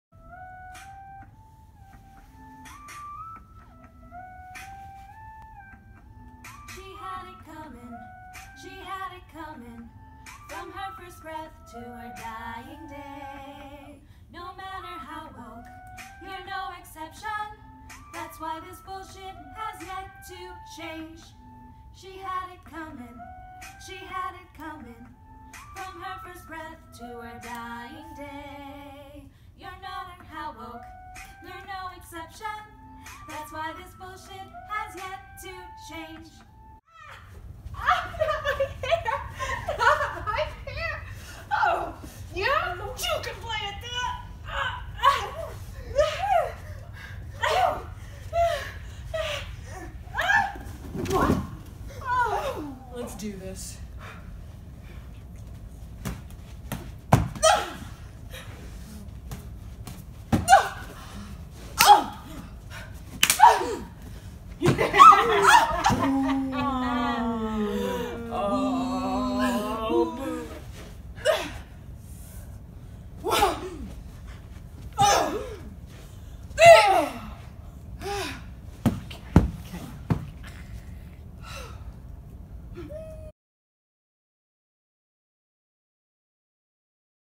A Second Peep at PEEPSHOW Rehearsals
PEEPSHOW started rehearsal on January 4, 2018 and here's a look into what we've been up to. What's PEEPSHOW? It's dog & pony dc's newest production, our entry into the 2018 WomensVoices Theatre Festival. Feb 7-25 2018.

PEEPSHOW takes-back objectification and sexism, and kicks the patriarchy where it counts. Over PEEPSHOW’s five spectacles, performers will review the four waves of feminism in a wrestling ring, explore the complexities of the male gaze through Broadway show tunes, and expose the modern woman’s complicity in her own oppression through a synchronized-swimming-and Kpop-inspired-ballet.

There isn’t another offering as part of Washington, DC’s second Women’s Voices Theatre Festival with a multi-sensory ode to female genitalia, which means PEEPSHOW is not to be missed.

Recommended for 18yo+. 100% accessible to and enjoyable by hearing and Deaf audience members. PEEPSHOW is an intimate experience, capped at 30 persons per showing.